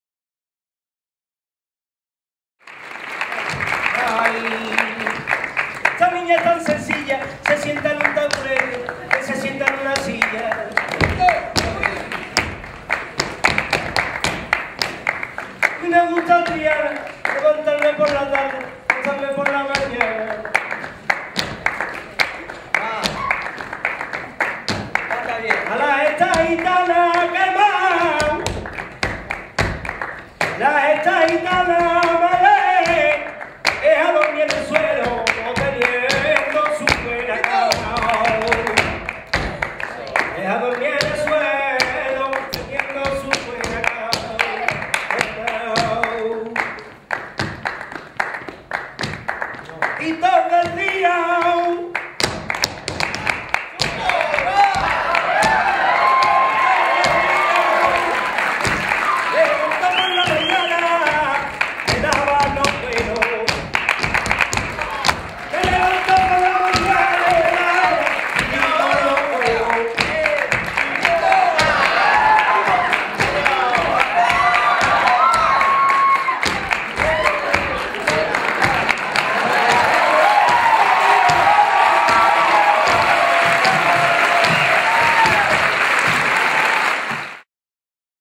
Fiesta por Bulerias: Fiona Malena Flamenco Ensemble & Juan Murube
Fiona Malena Flamenco Ensemble in "Inspiración." Cantaor: Juan Murube